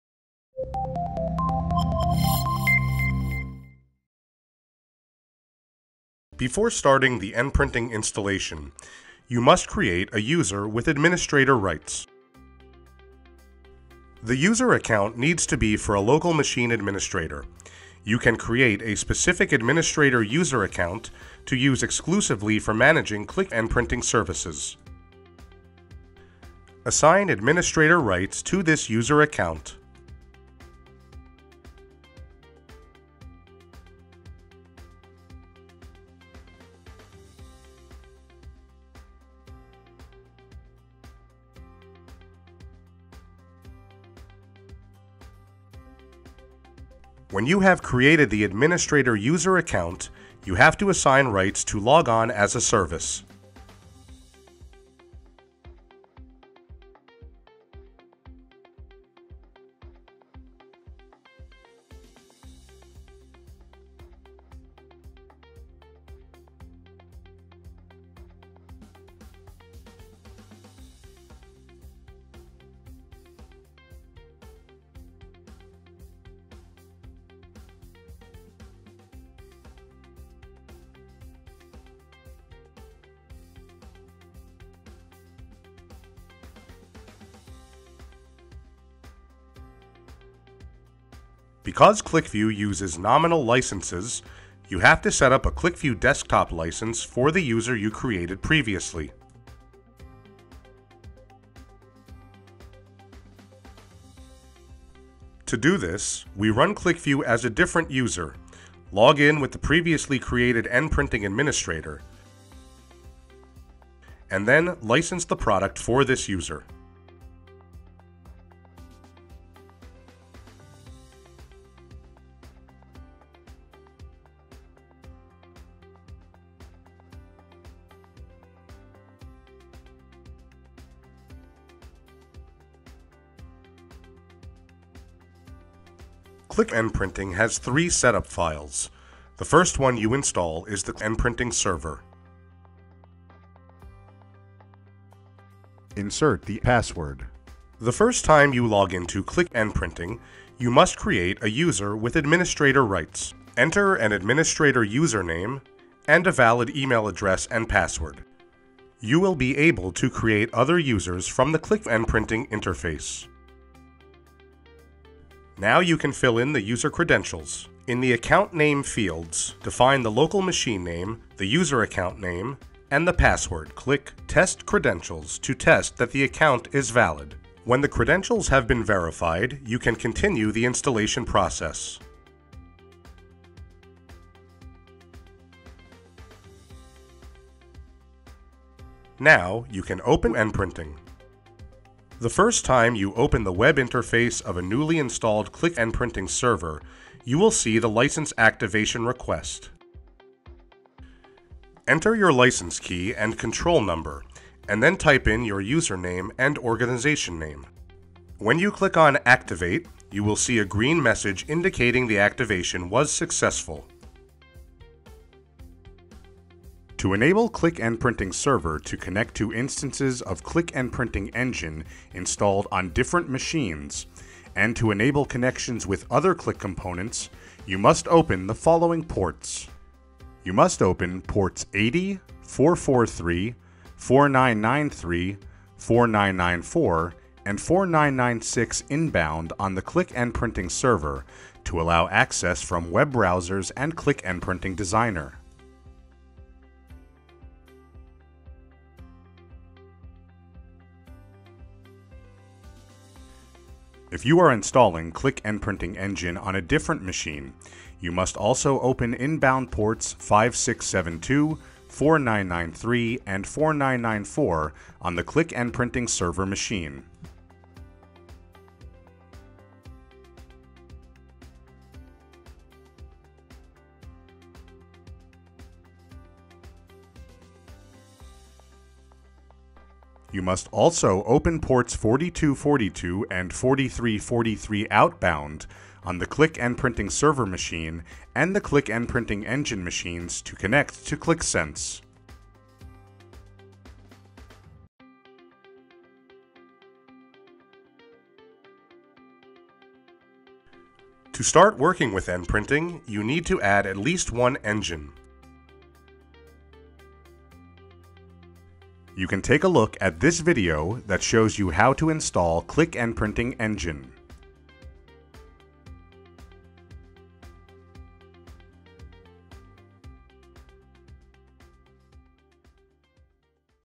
Installing Qlik NPrinting Server
This video show you how to setup the environment to allow Qlik NPrinting to connect to Qlik Sense and QlikView and how to install Qlik NPrinting Server and activate a license for the administrator user.